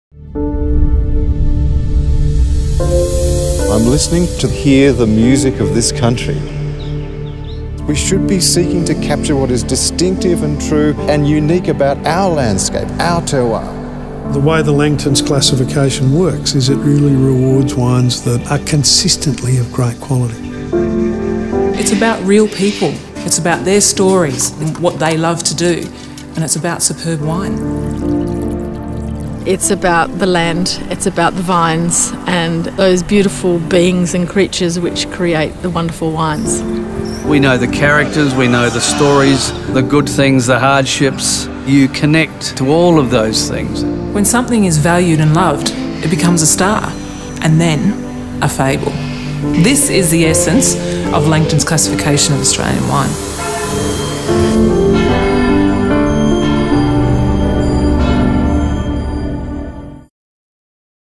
LANGTONS CLASSIFICATION 1MIN TRAILER 2014
Shortened 1 minute trailer for our Classification VI release at Tasting Australia in Adelaide on the 1st May 2014.